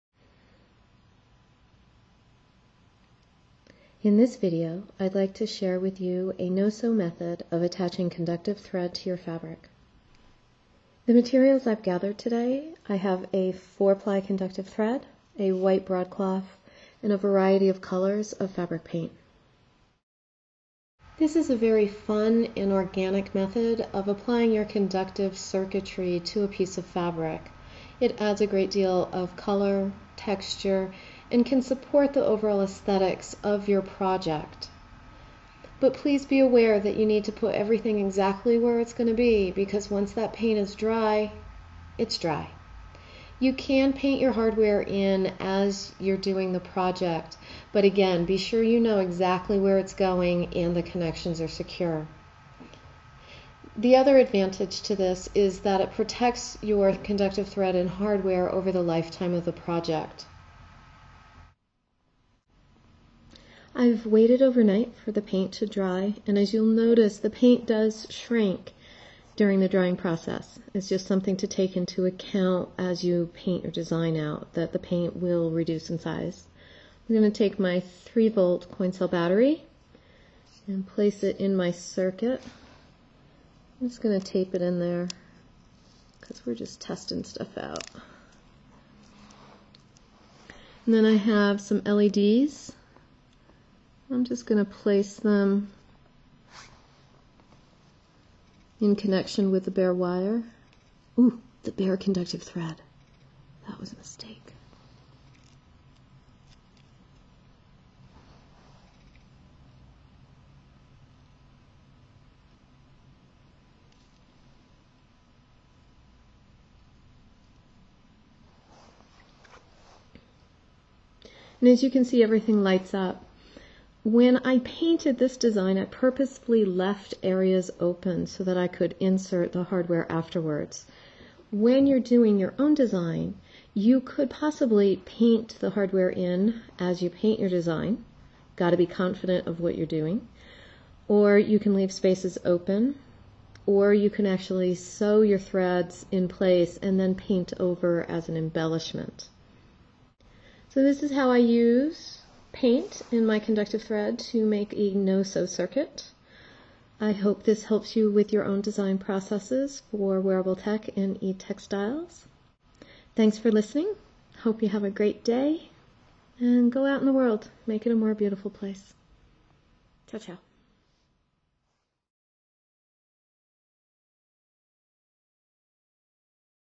Paint Covering Conductive Thread
Apply conductive thread to fabric by using fabric paint.  This method not only adheres the thread to the fabric it supports your design aesthetic and protects the circuitry of your wearable tech or eTextile project.